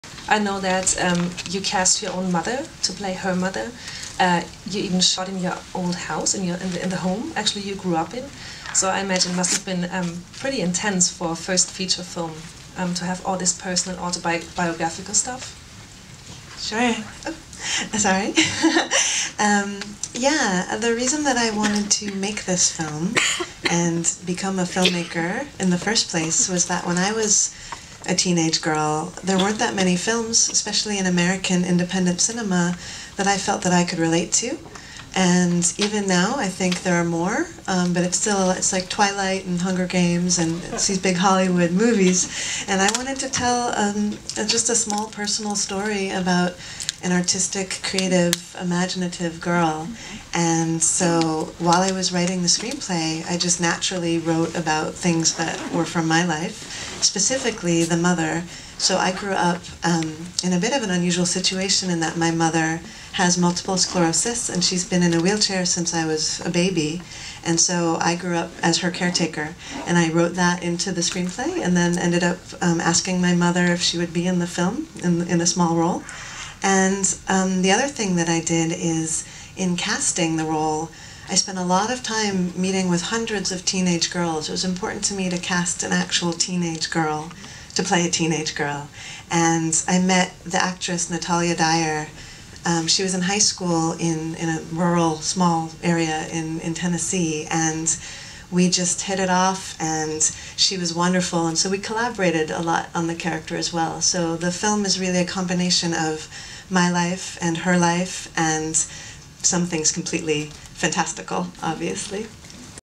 Behind the Sunglasses | Q&A Leah Meyerhoff - I BELIEVE IN UNICORNS
Regisseurin Leah Meyerhoff spricht über ihr kleines, persönliches Spielfilmdebüt I BELIEVE IN UNICORNS und die Verbundenheit der Story zwischen ihrem eigenen und dem Leben ihrer Hauptdarstellerin.